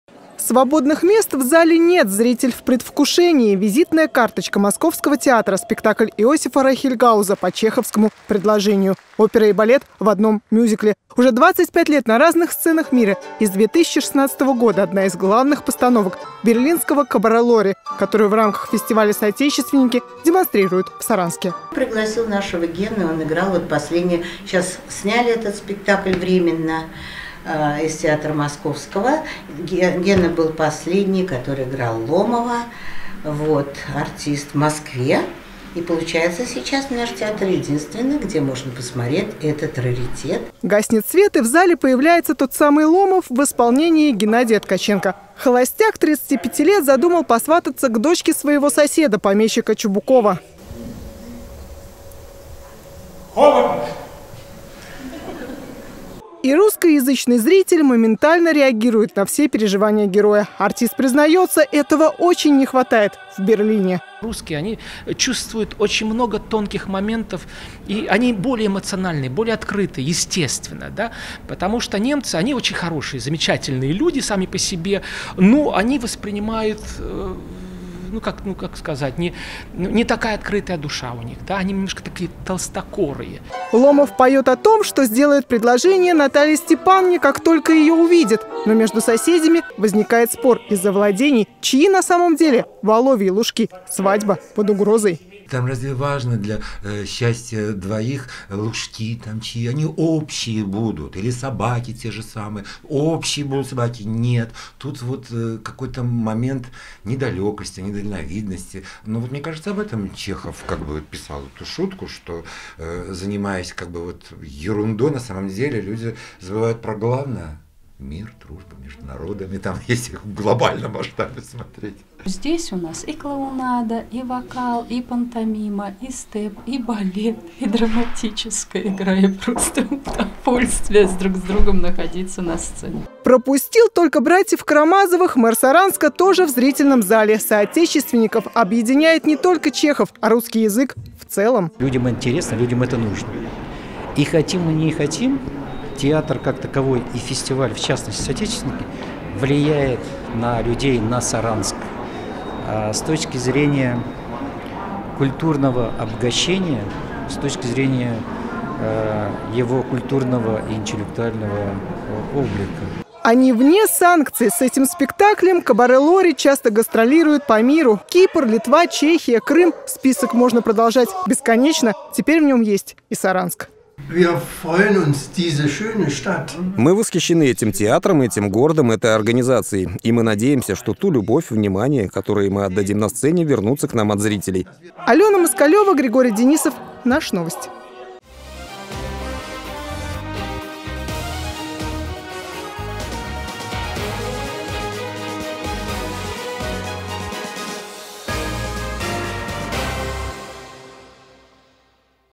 Чехов объединяет «Соотечественников»
«А чой-то ты во фраке?!?». История о том, как не остаться в холостяках и как земельный вопрос может помешать женитьбе. Знаменитый спектакль по пьесе-шутке Антона Чехова «Предложение» в исполнении берлинского театра «Кабаре Лори». В пятый день 14-го международного фестиваля русских драматических театров на саранской сцене уникальные артисты Светлана Лучко, Вадим Граковский и Геннадий Ткаченко-Папиж.